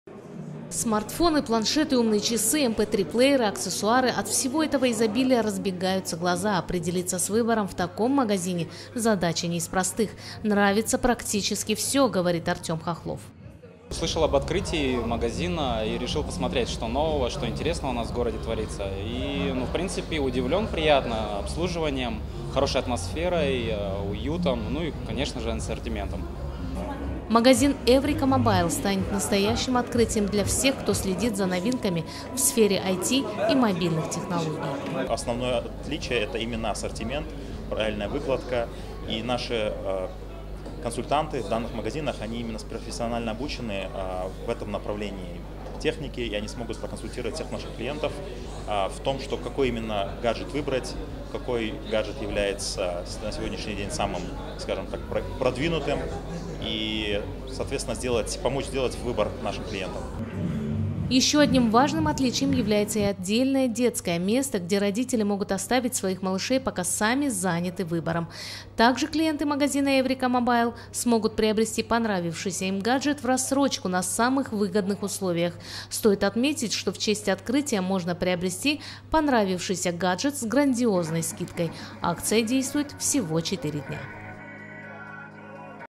В Шымкенте открылся магазин "Evrika mobile"
Концептуально новый специализированный магазин по продаже мобильных гаджетов открыт в Шымкенте. В широчайшем ассортименте можно найти самые последние образцы современных технологических решений в сфере мобильной, радио и аудио техники.